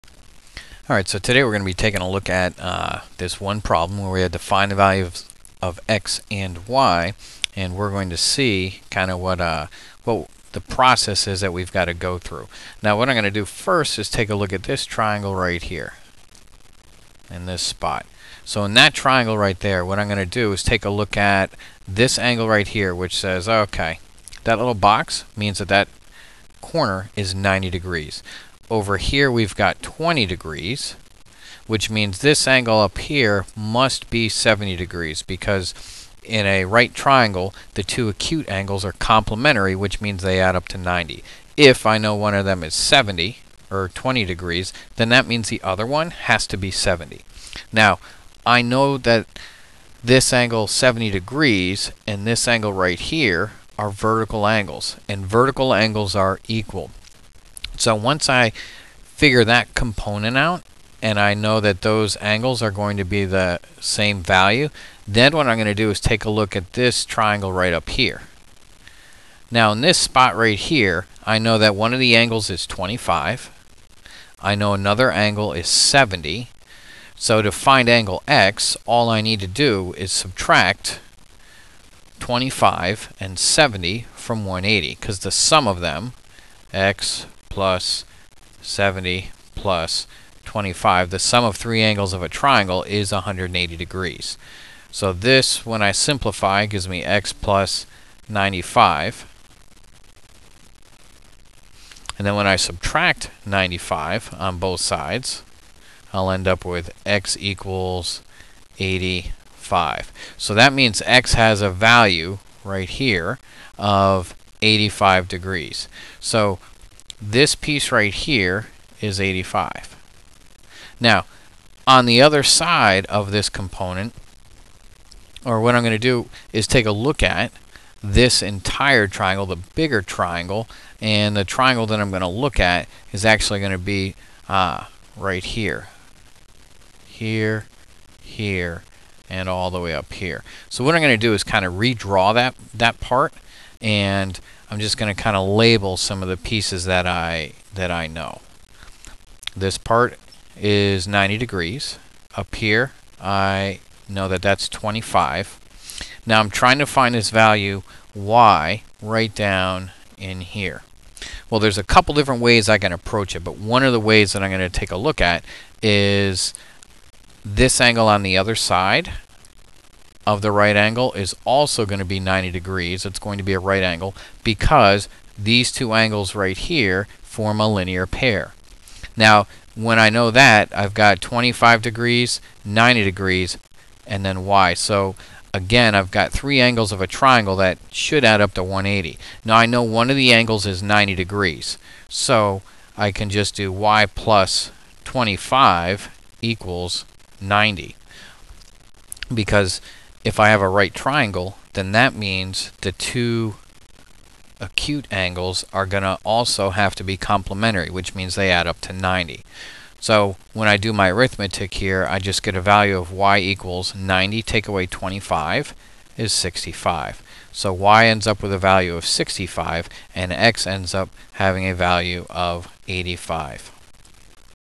4-1 Triangle Properties Problem 35
Find the values of x and y for a given triangle problem. This problem involves linear pair angles as well as the Triangle Sum Theorem.